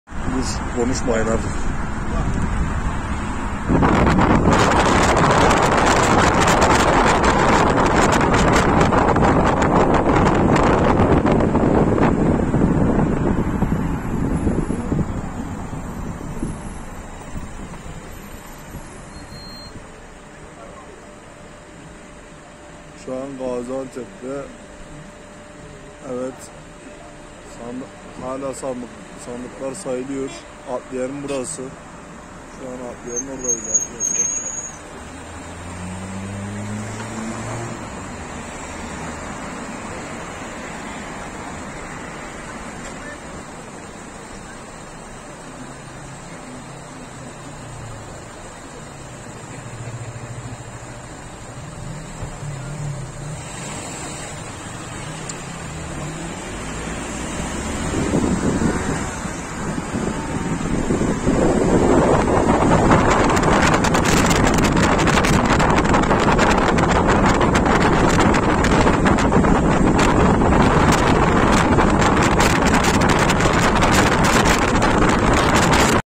GAZİANTEP ADLİYESİ OYLAR SAYILIYOR SEÇİMLER DAKİKALAR KALA LA İLAHE İLLALLAH RECEP TAYİP ERDOĞAN
RECEP TAYYİP ERDOĞAN
KEMAL KILIÇDAROĞLU
Dr. SİNAN OĞAN
seçim sonuçları
# seçim
türkiyede seçim